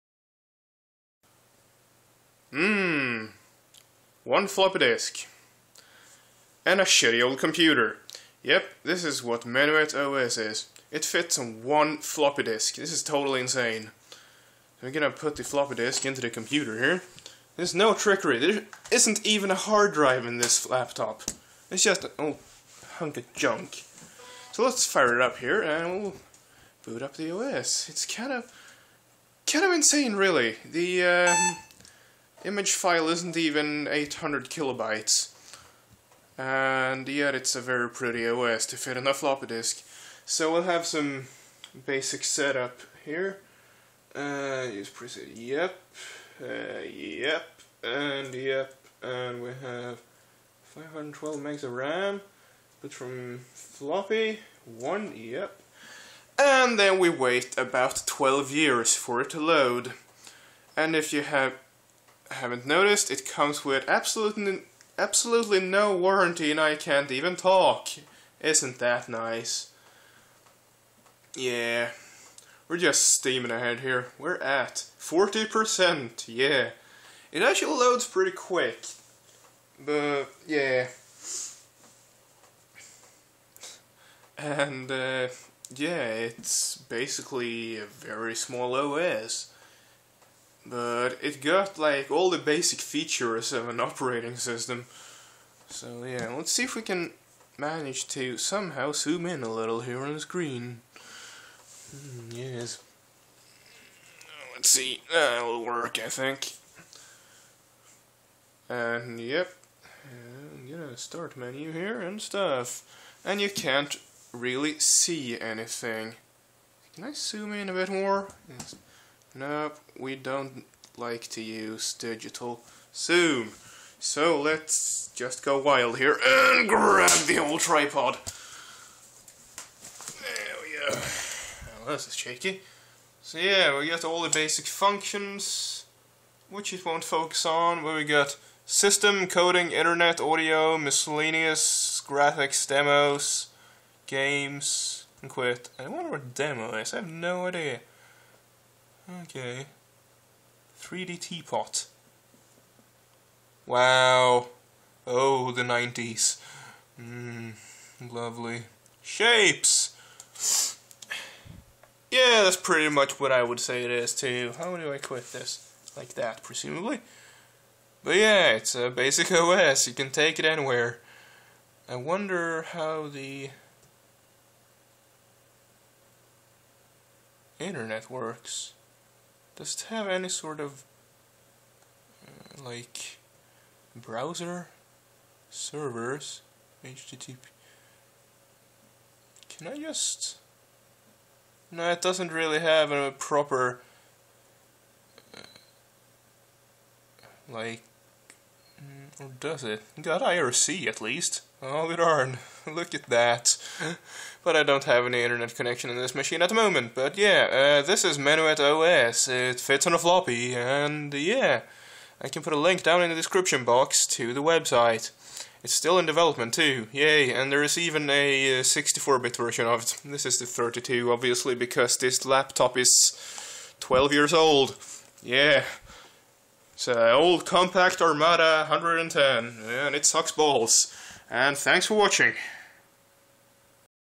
MenuetOS floppy based OS
A pretty neat little OS that fits on a single floppy disk.